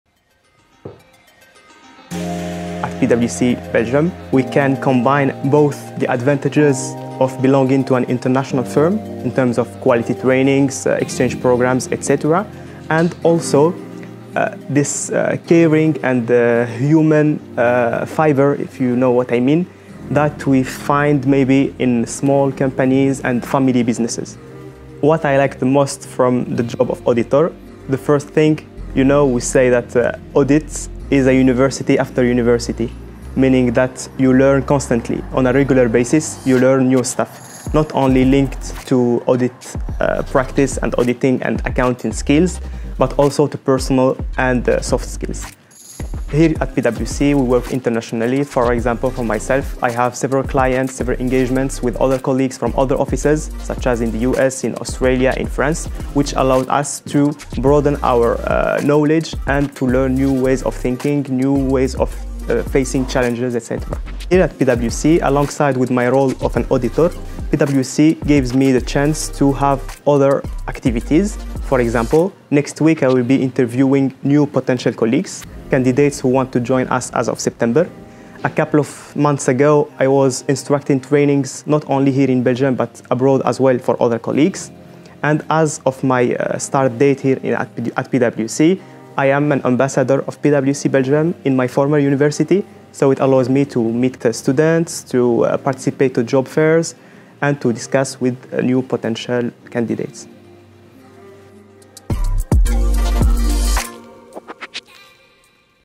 Get the best of both worlds
El Ghali Mokhantar is a senior associate at PwC. He says he gets the best of both worlds at PwC Belgium: the growth opportunities, global networks and expertise of an international business, combined with the caring and friendly atmosphere that can be found in small companies and family businesses. “Audit is like a university after university,” says El Ghali. “You are constantly learning, not only audit and accounting skills, but also personal and soft skills.” He also appreciates the international aspect of life at PwC: “you learn new ways of thinking and new ways of facing challenges.”